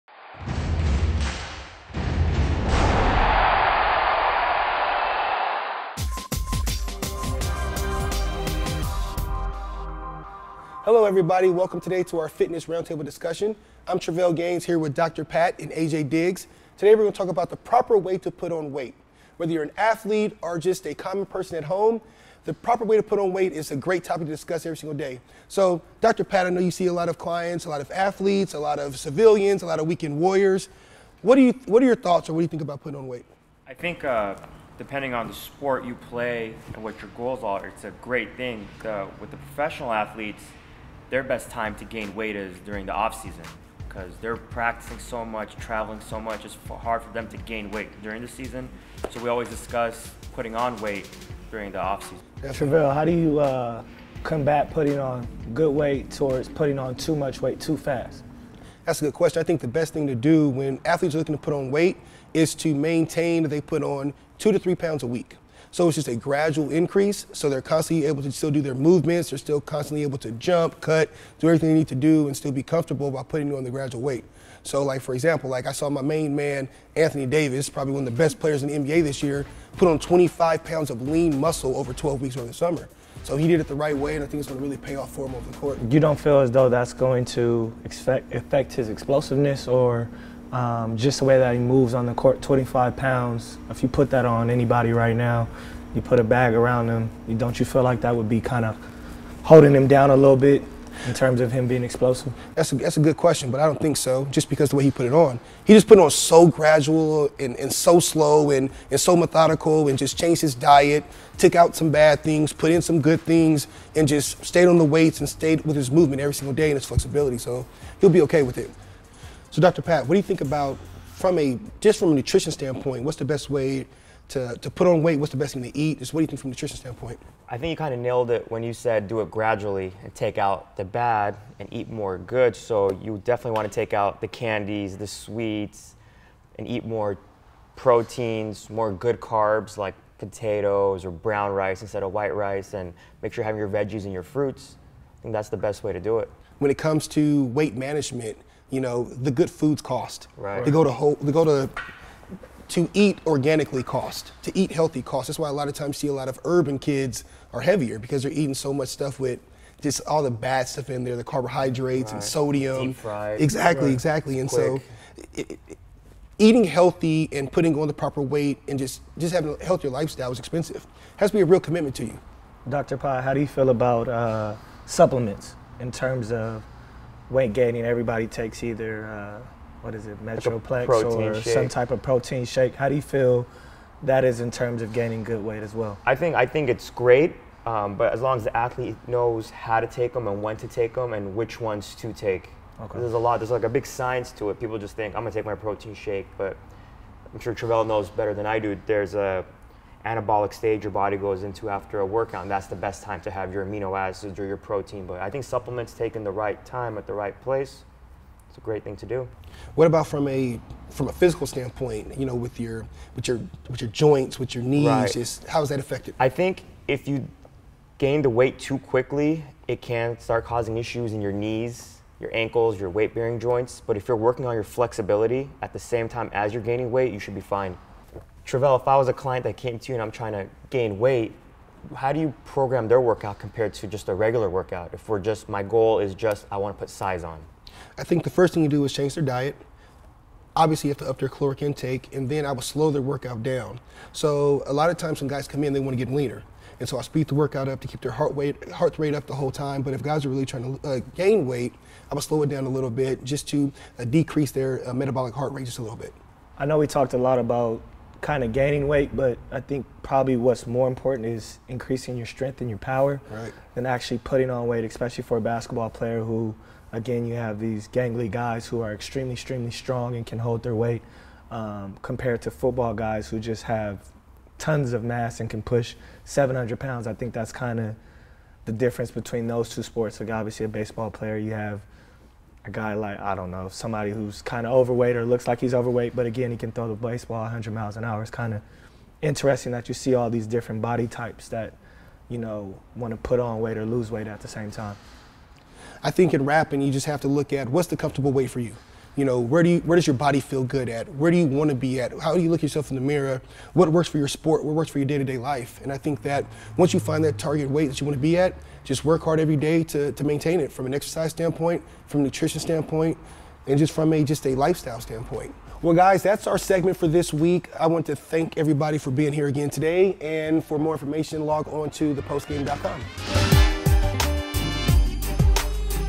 Fitness Roundtable: The Proper way to Gain Weight
Our panel of fitness experts tell you some keys to remember when trying to gain weight.  Learn the most important part of putting on pounds and how some pro athletes have been able to gain weight and keep their athleticism in top form.